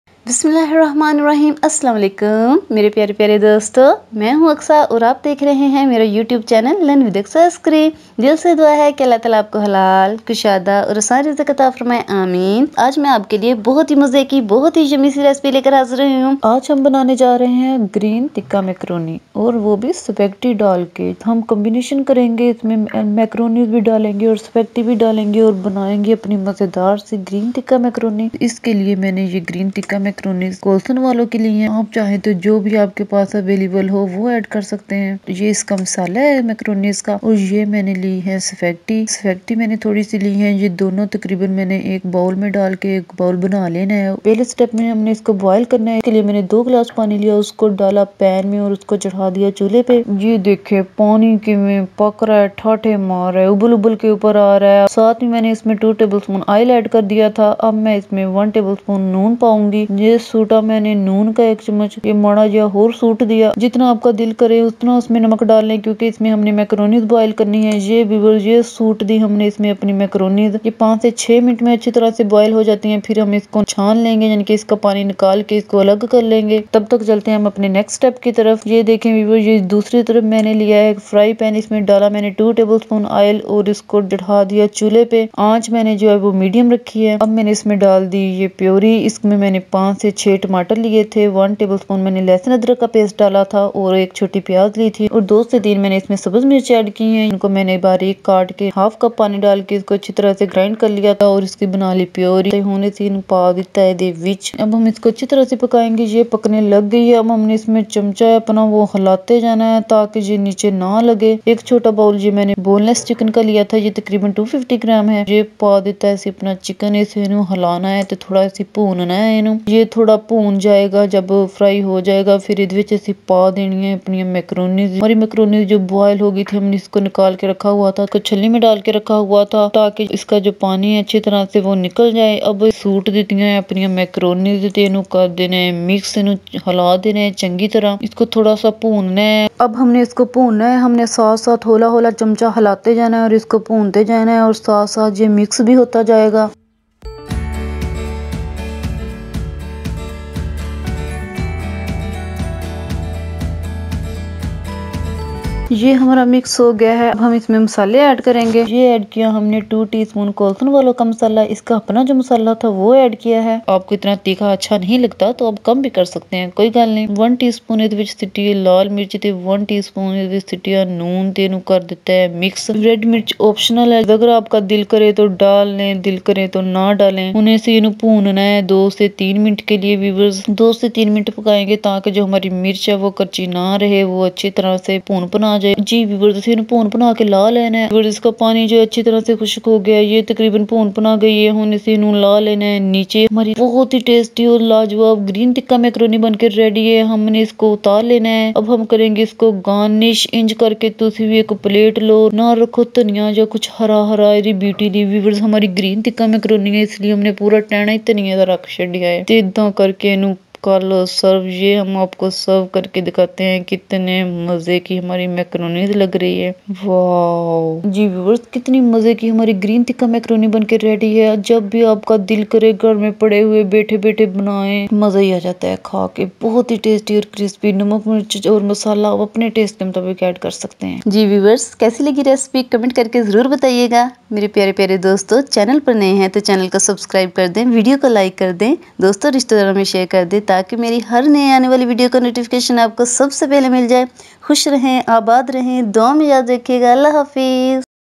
Macaroni|چکن تکہ میکرونی|Green Tikka Macaroni,Chicken Macaroni, How to make macaroni by Aqsa Askari
Assalam-o-Alaikum Everyone, Today m going to share with u a recipe of tikka macaroni.

In this channel Learn With Aqsa Askari I share with u:
Skin care
Health tips
Cooking Tips
Plants Care